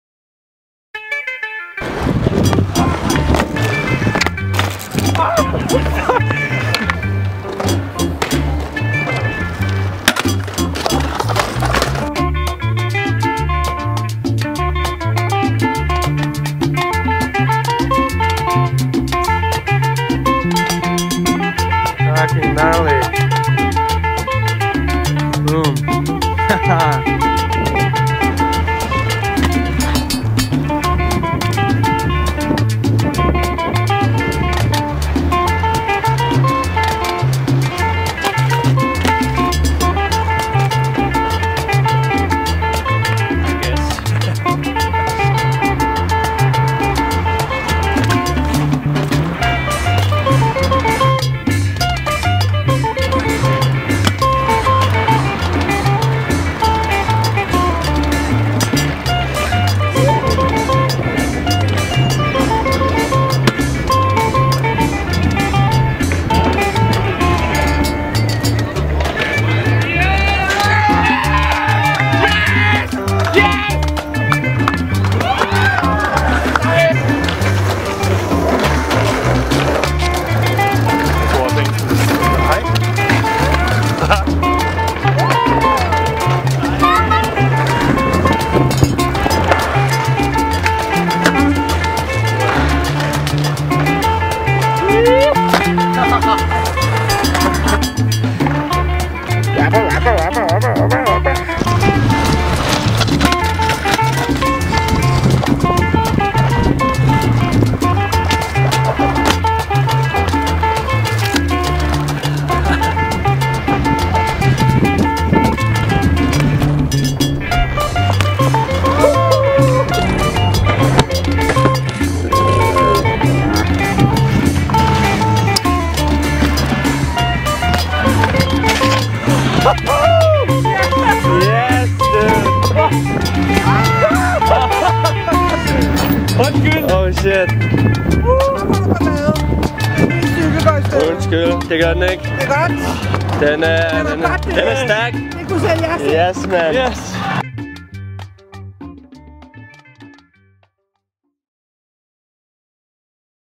Dragun y Nissen - Bicicletas
A little video from Copenhagen featuring Konstantin Dragun y Johan Nissen

Filmed by: Konstantin, Johan, Marcus Diemar and Christian Tejnø
Edited by: Johan Nissen